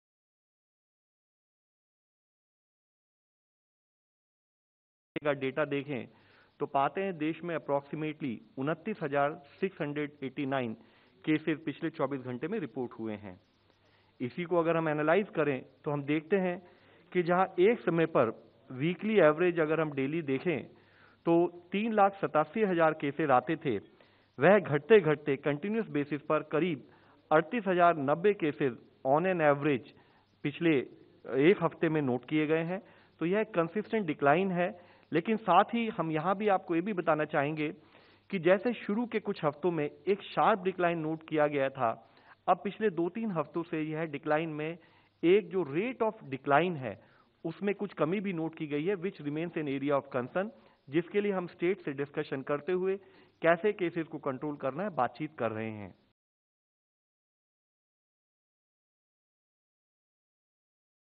Rate of decline in COVID cases remains area of concern: Health Ministry
New Delhi, July 27 (ANI): The Joint Secretary of Health Ministry, Lav Agarwal on July 27 said that the rate of decline in the COVID cases from past 2-3 weeks remains the area of concern. “There has been a consistent decline in the weekly average in the COVID cases. But if we compare the rate of decline in cases, from earlier to now, its decrease remains the area of concern. We are in talks with states in this regard.” (SA)